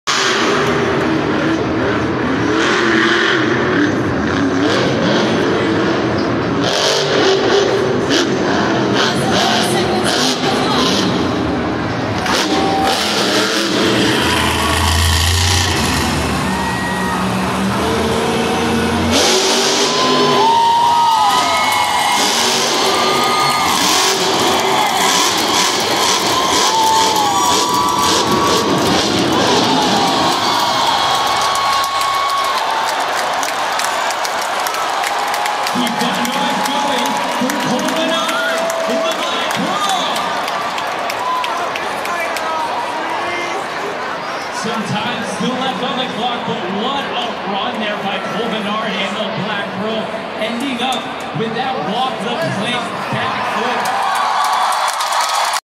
FREESTYLE BACKFLIP & WHEELIE // MONSTER JAM
The ultimate in monster truck racing, as these mechanical beasts compete against each other.  At the event, you will witness world-class driver athletes and the most recognizable trucks tear up the dirt and compete in intense competitions of speed and skill.